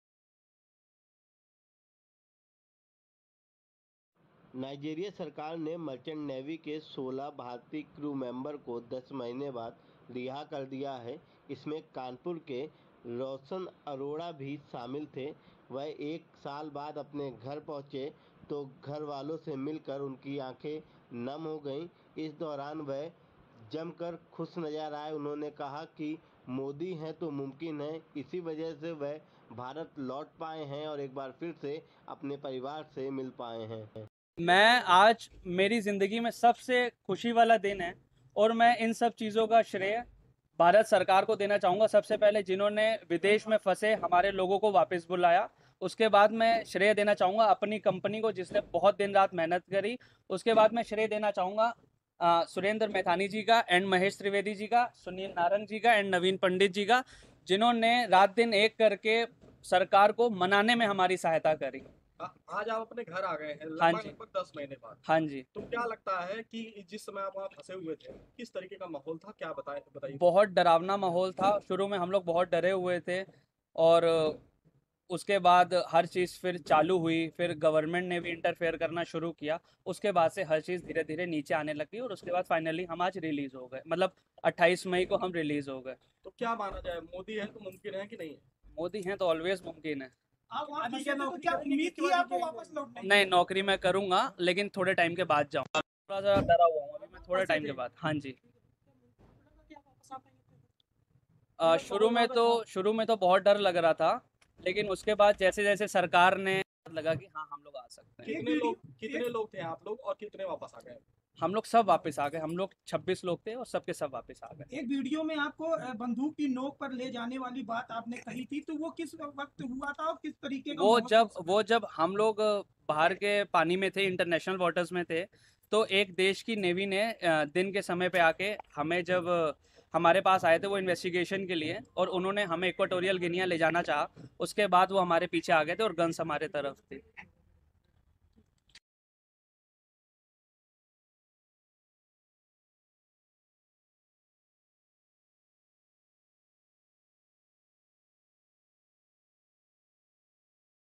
Kanpur: सात समुंदर पार की हिरासत से 10 माह बाद छूटकर आया रोशन, परिवार ने जताया मोदी सरकार का आभार
Kanpur News:  कानपुर का रोशन मर्चेंट नेवी में नौकरी करता था. वह अपनी शिपमेंट के साथ नाइजीरिया गया था. जहां पर उसकी शिप को बंदी बना लिया गया. करीब 10 महीने बाद अब नाइजीरिया से 16 भारतीय क्रू मेंबर से रिहा हुए हैं. जिसमें कानपुर का रोशन भी शामिल है. यहां देखें वीडियो